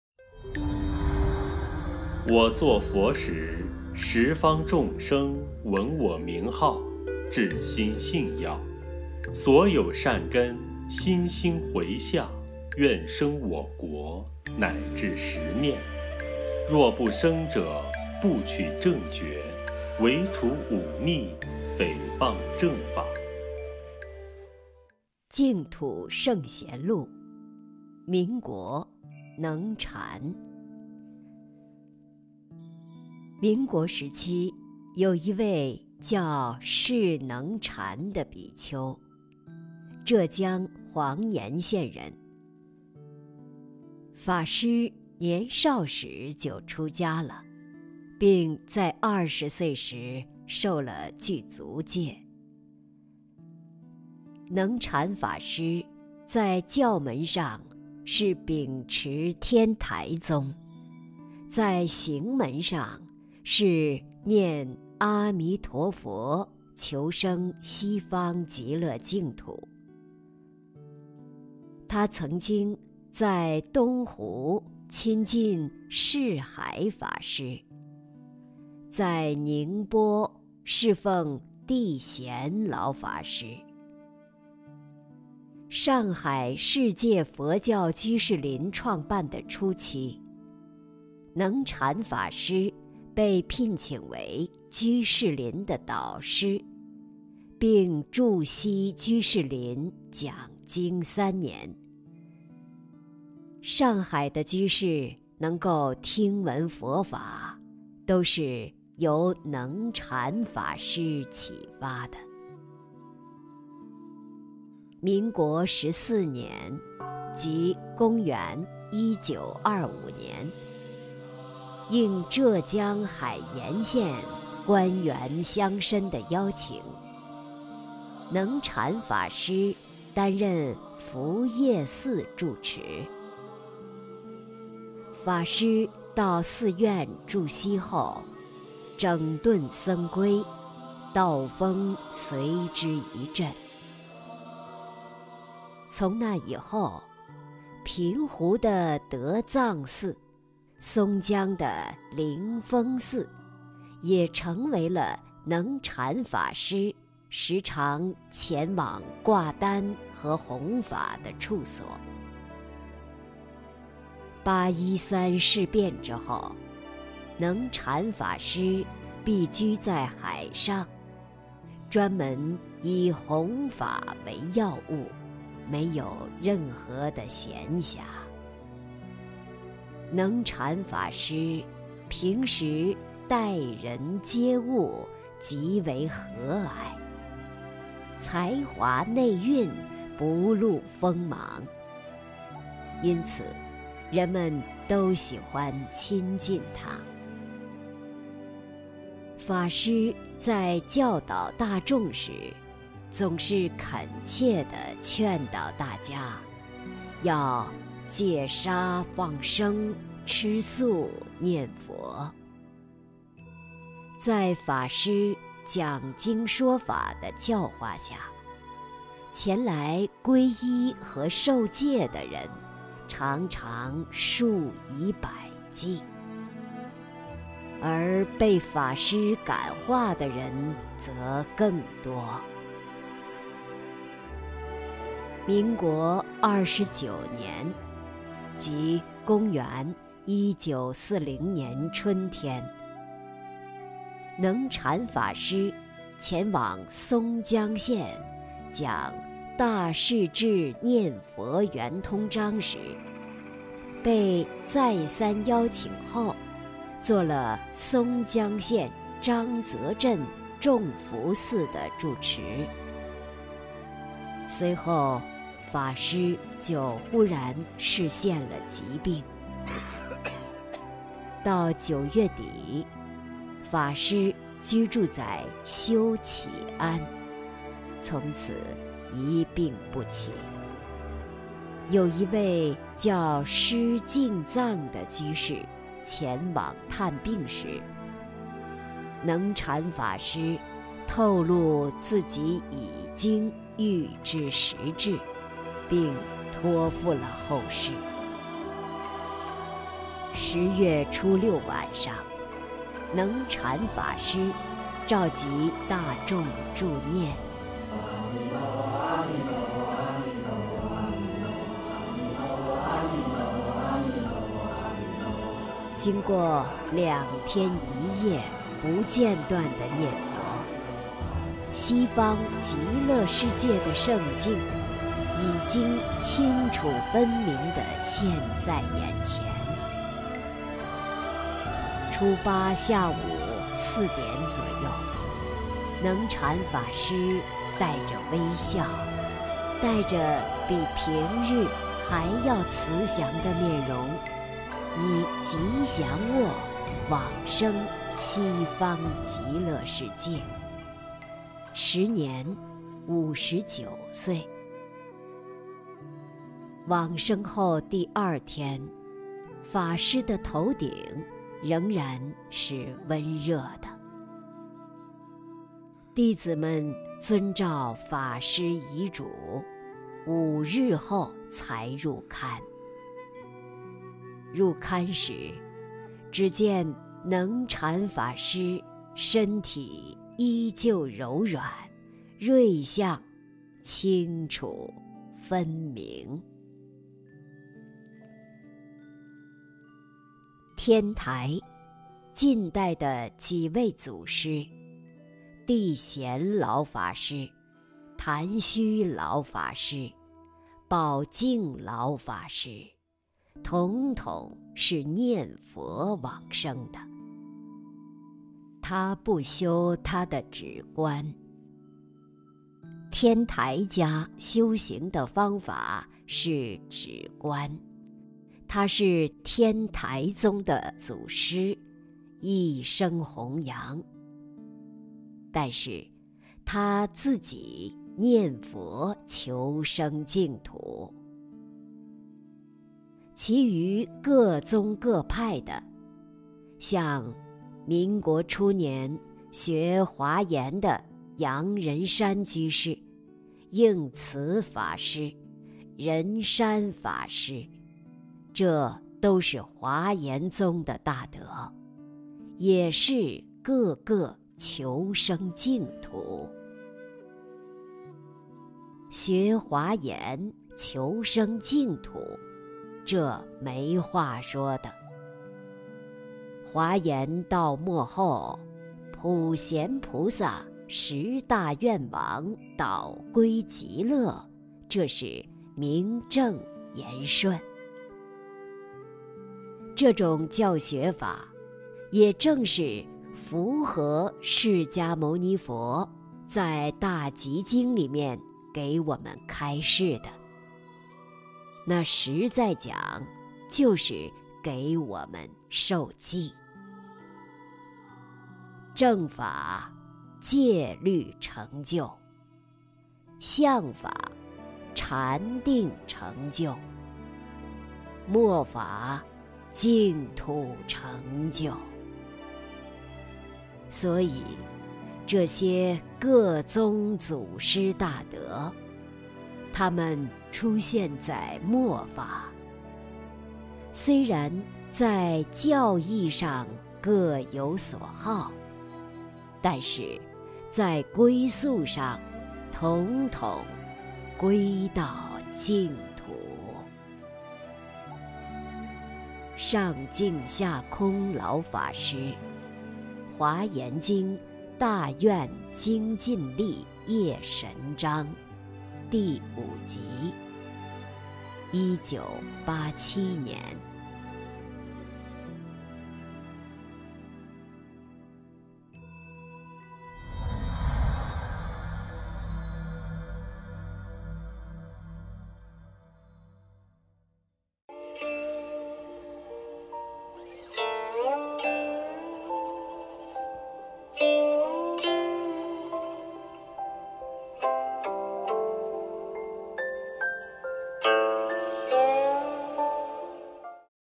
【淨土聖賢錄】 民國 能禪法師 - 高清 HD1080P
民國時期有一位，叫釋能禪的比丘。浙江黃巖縣人，法師年少時就出家了，
並在二十歲時受了具足戒。能禪法師在教門上是秉持天台宗，在行門上是念阿彌陀佛，求生西方極樂淨土。他曾經在東湖親近式海法師，在寧波侍奉諦閑老法師。上海世界佛教居士林創辦的初期，能禪法師被聘請為居士林的導師，並駐錫居士林講經三年。上海的居士能夠聽聞佛法，都是由能禪法師啟發的。

民國十四年，即西元1925年，應浙江海鹽縣官員鄉紳的邀請，能禪法師擔任福葉寺住持。法師到寺院駐錫後，整頓僧規，道風隨之一振。從那以後，平湖的德藏寺，松江的靈峰寺，也成為了能禪法師時常前往掛單和弘法的處所。

「八一三事變」之後，能禪法師避居在海上，專門以弘法為要務，沒有任何的閒暇。能禪法師平時待人接物，極為和藹，才華內蘊，不露鋒芒，因此人們都喜歡親近他。法師在教導大眾時，總是懇切的勸導大家要戒殺放生，吃素念佛。在法師講經說法的教化下，前來皈依和受戒的人常常數以百計，而被法師感化的人則更多。

民國二十九年，(即西元1940年)春天，能禪法師前往松江縣講《大勢至念佛圓通章》時，被再三邀請後做了松江縣張澤鎮種福寺的住持，隨後法師就忽然示現了疾病。到九月底，法師居住在修啟庵，從此一病不起。有一位叫施靜藏的居士前往探病時，能禪法師透露自己已經預知時至，並託付了後事。十月初六晚上，能禪法師召集大眾助念，經過兩天一夜不間斷的念佛，西方極樂世界的聖境，已經清楚分明的現在眼前。初八下午四點左右，能禪法師帶著微笑，帶著比平日還要慈祥的面容，以吉祥臥往生西方極樂世界! 時年五十九歲，往生後第二天，法師的頭頂仍然是溫熱的，弟子們遵照法師遺囑，五日後才入龕。
入龕時只見能禪法師身體依舊柔軟，瑞相清楚分明。